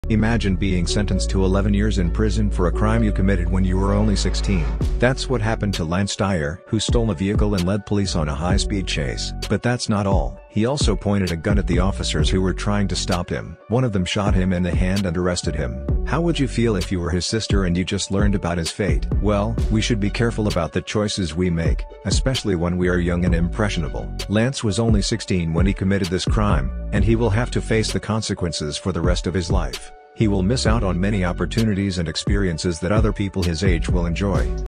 Lance Dyer’s Mistake: 11 Years in Jail for a 16-Year-Old
Imagine being sentenced to 11 years in prison for a crime you committed when you were only 16. That’s what happened to Lance Dyer, who stole a vehicle and led police on a high-speed chase. But that’s not all. He also pointed a gun at the officers who were trying to stop him. One of them shot him in the hand and arrested him. How would you feel if you were his sister and you just learned about his fate? well, We should be careful about the choices we make, especially when we are young and impressionable. Lance was only 16 when he committed this crime, and he will have to face the consequences for the rest of his life. He will miss out on many opportunities and experiences that other people his age will enjoy.